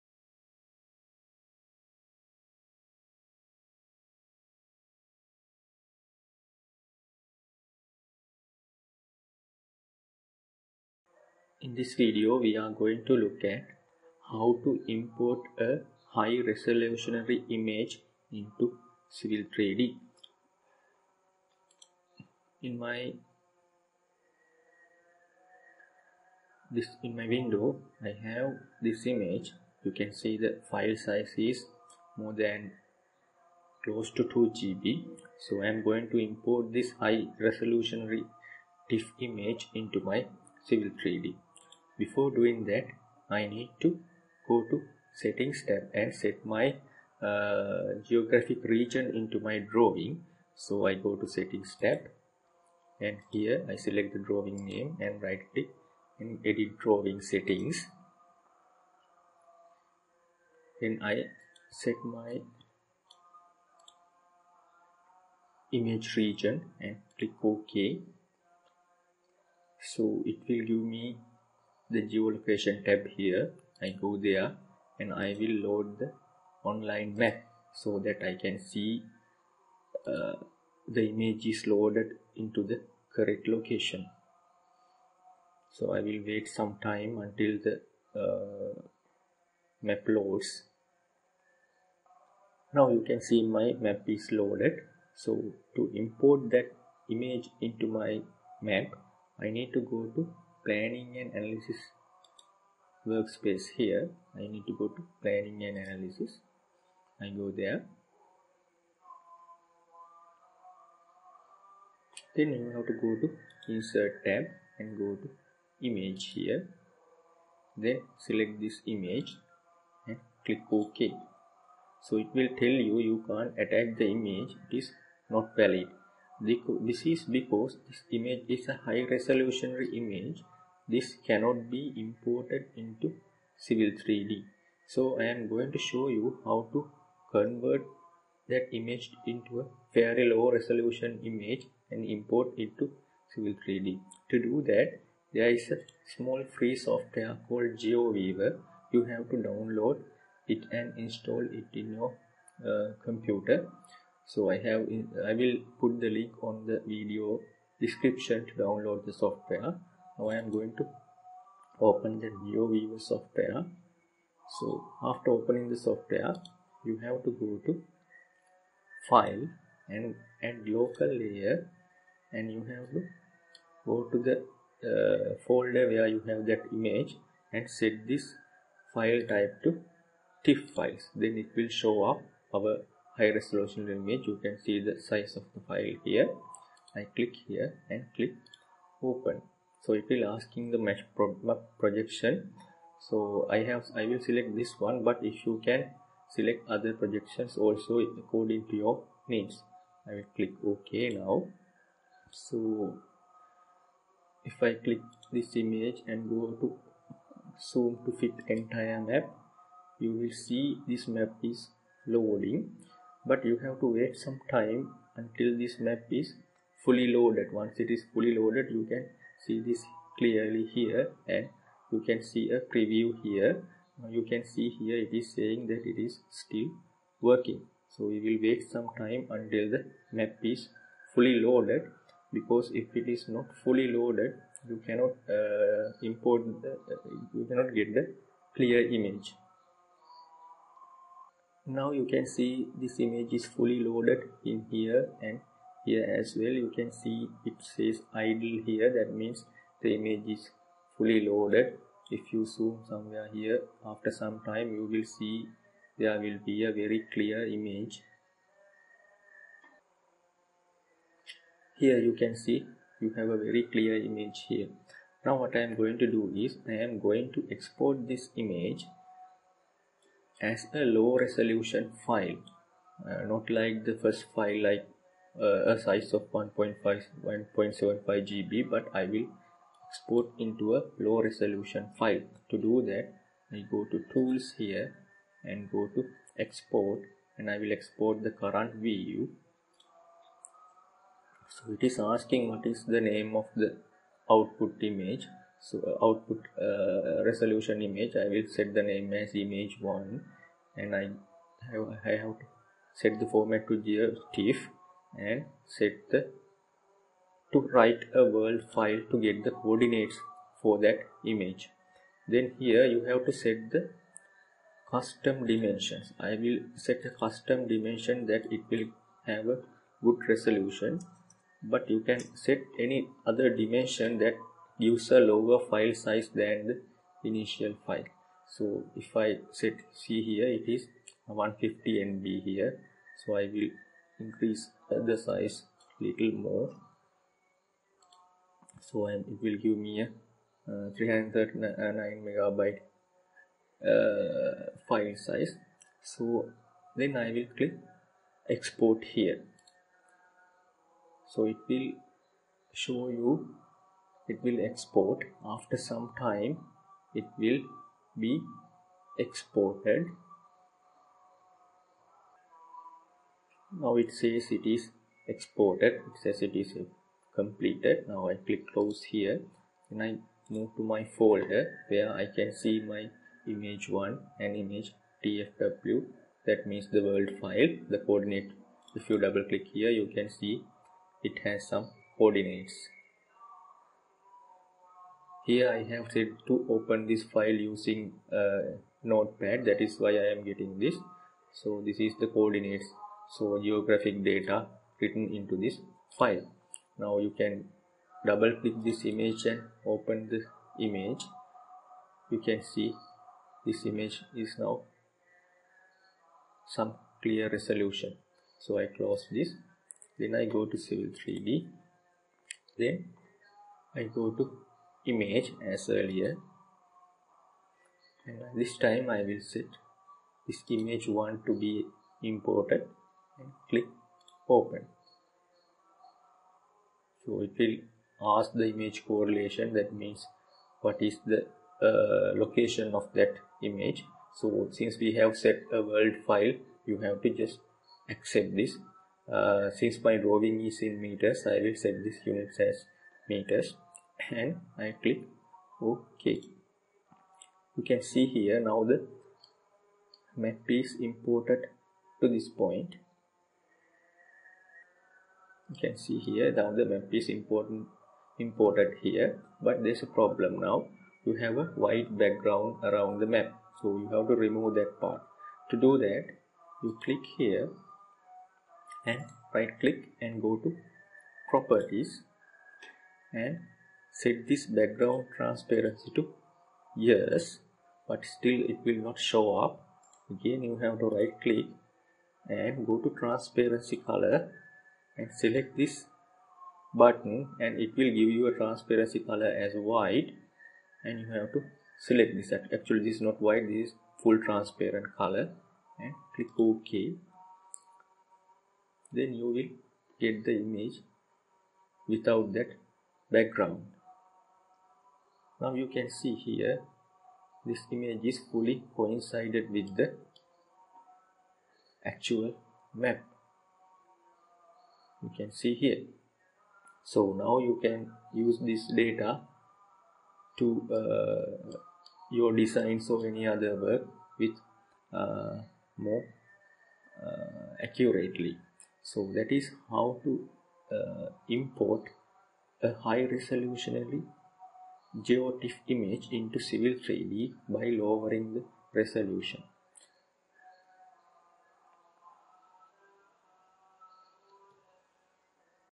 Insert High Resolution TIFF Image in to Civil 3D
When trying to import a Tiff image into AutoCAD or an AutoCAD-based product such as AutoCAD Civil 3D, one of the following errors is displayed:

Error: "Image is Invalid"
"Cannot be found or invalid format"
"Tif was not found or is not valid"

In some cases, the program displays the following in the command line, but fails to insert into the drawing:

"Enter image file name to attach"

Ref-Autodesk Knowledge Network

This video shows you how to resolve this issue using free software Geoviewer.

For Clarification & Training related to Civil 3D Road Design